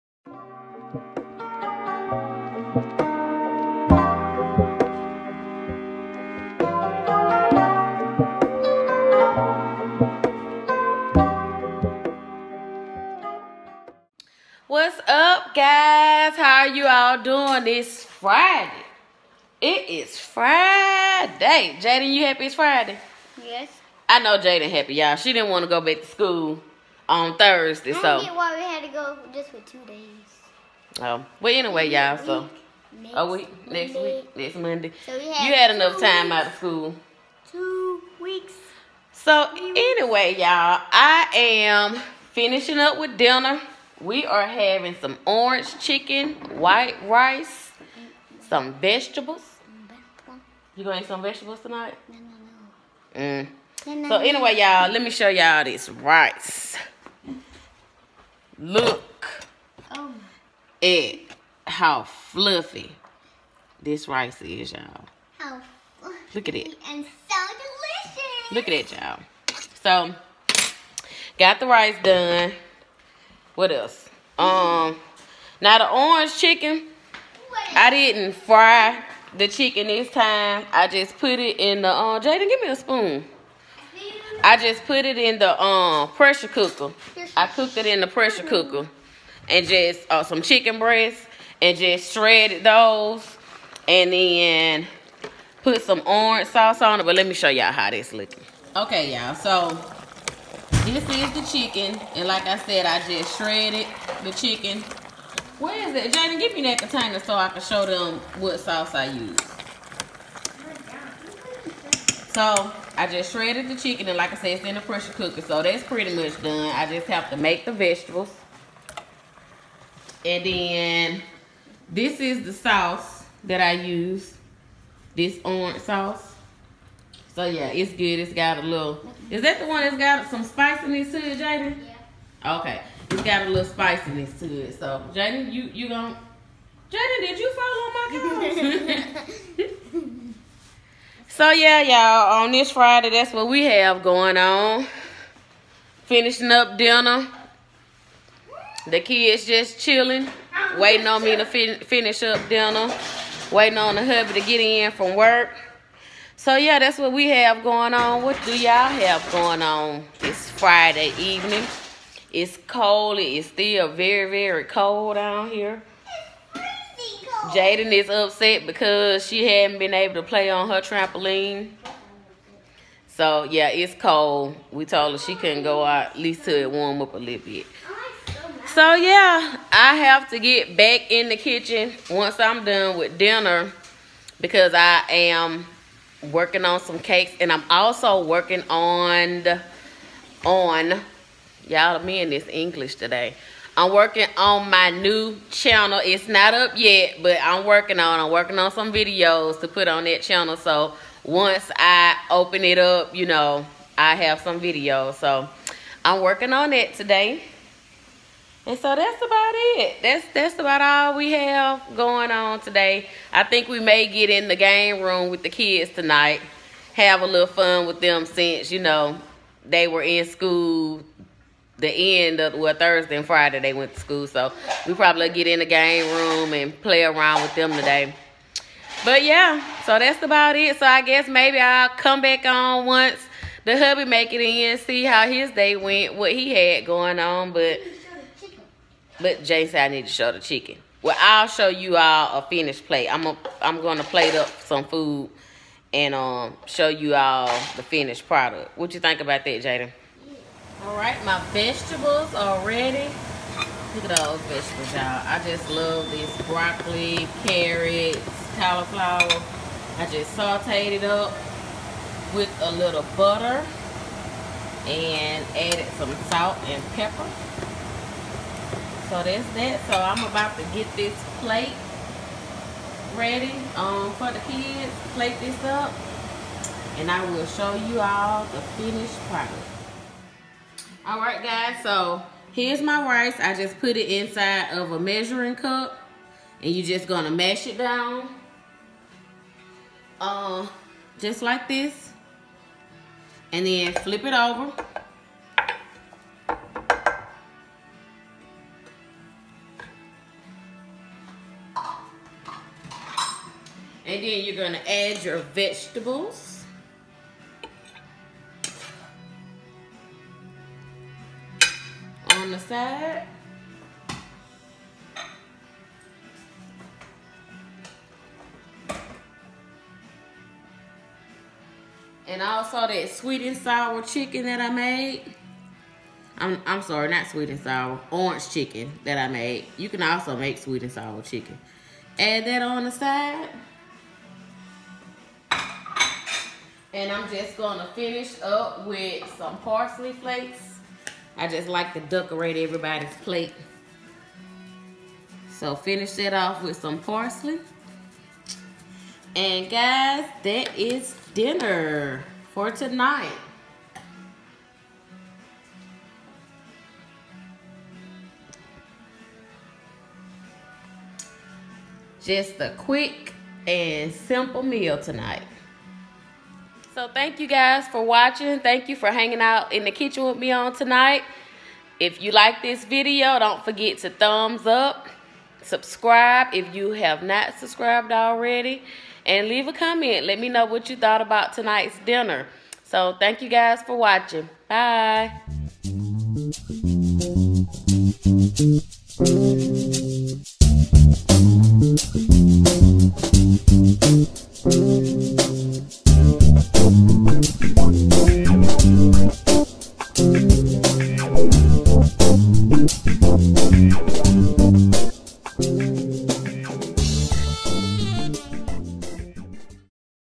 30 MINUTE EASY MEAL | FAMILY VLOGS
Dinner tonight was quick and easy, I made Orange chicken in the pressure cooker. Along with some white rice and vegetables. Enjoy!